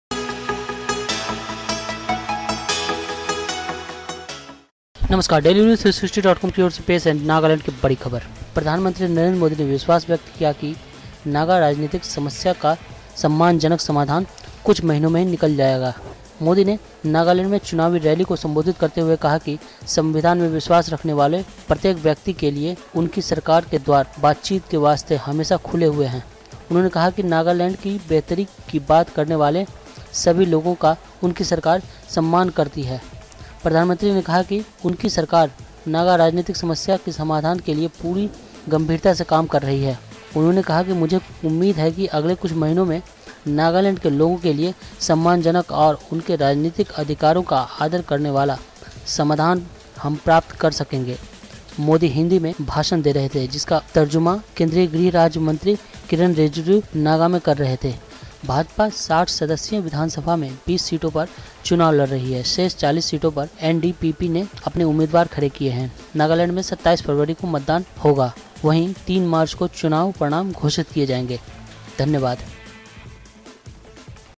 PM मोदी ने कहा, 'नागा समस्या का जल्द निकलेगा समाधान'
प्रधानमंत्री नरेन्द्र मोदी ने गुरुवार को विश्वास व्यक्त किया कि नागा राजनीतिक समस्या का सम्मानजनक समाधान कुछ महीनों में ही निकाल लिया जायेगा। मोदी ने यहां चुनावी रैली को संबोधित करते हुए कहा कि संविधान में विश्वास रखने वाले प्रत्येक व्यक्ति के लिए उनकी सरकार के द्वार बातचीत के वास्ते हमेशा खुले हुए हैं।